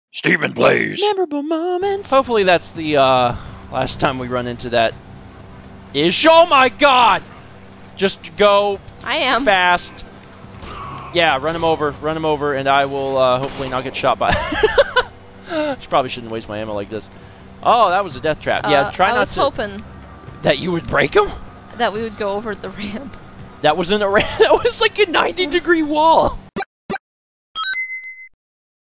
Ramp Wall
If the game won't give Mal a ramp, she'll just make her own.